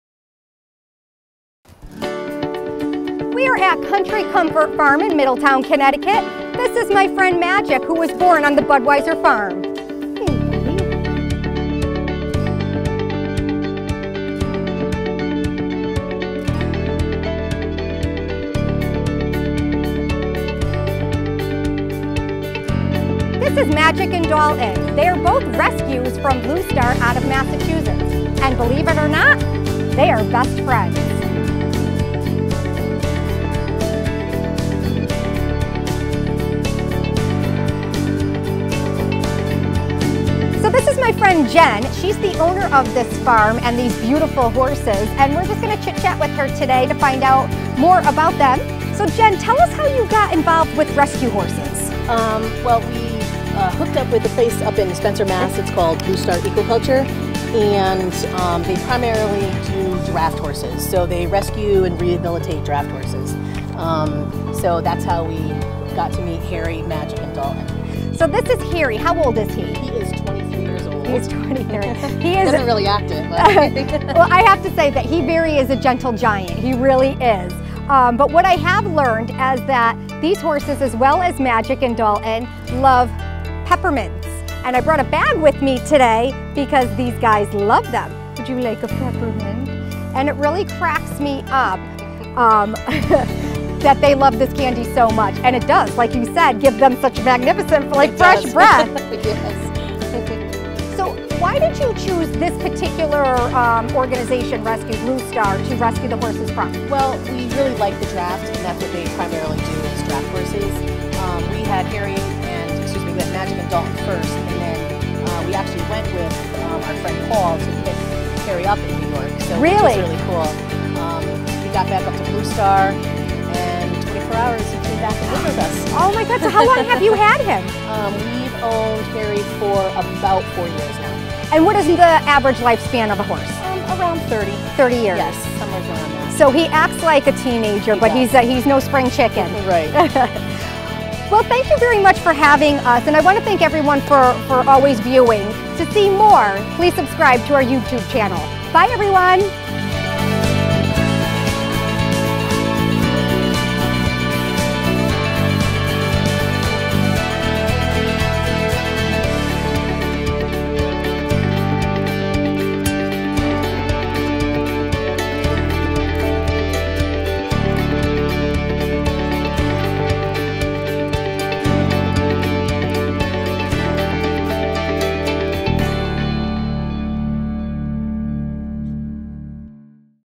Country Comfort Farm | Allyson Genovese Mrs. Connecticut America 2016
Mrs. Connecticut America 2016 Allyson Genovese drops in at Country Comfort Farm located in Middletown, CT to visit with the Clyde's Dale Horses that owners Jennifer and Jim Cosenza have on the farm. Ally learns all about the horses, the draft rescue program to which the Cosenza's obtain the horses and what each horse is like. See what adventure Allyson had on her day out on the farm!

Featuring: Allyson Genovese and Jennifer Cosenza
Photos Provided By: Christine Petit
Video Produced By: MV Film Productions